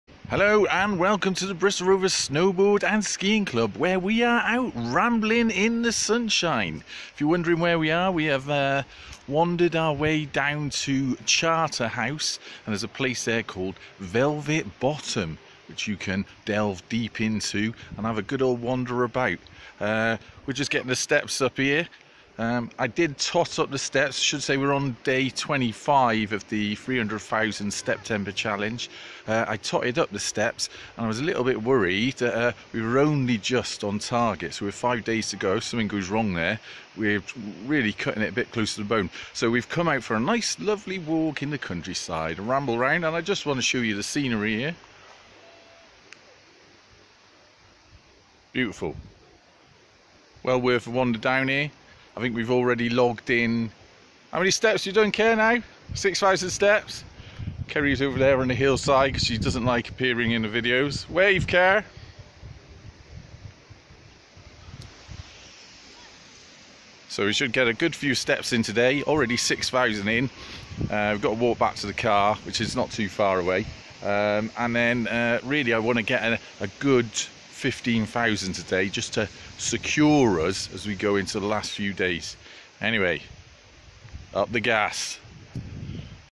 FITNESS - STEPtember. Day 25
We're nearing the end of this challenge and steps are lacking. Time for some desperate additional rambling. Jem also has a hat on. UTG